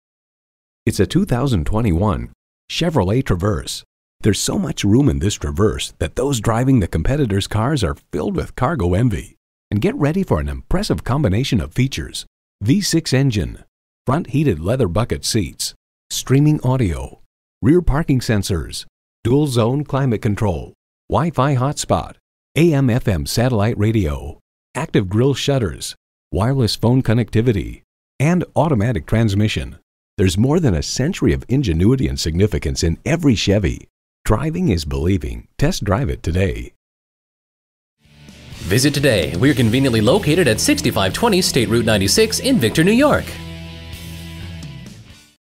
2021 Chevrolet Traverse Rochester NY Victor, NY
Year: 2021
Make: Chevrolet
Model: Traverse
Trim: LT Leather
Engine: 3.6L V6 SIDI VVT
Transmission: 9-Speed Automatic
Color: Gray
Interior: Jet Black/Dark Galvanized
Mileage: 65800
Stock #: VTO46998
VIN: 1GNEVHKW7MJ213591

Address:
6520 State Route 96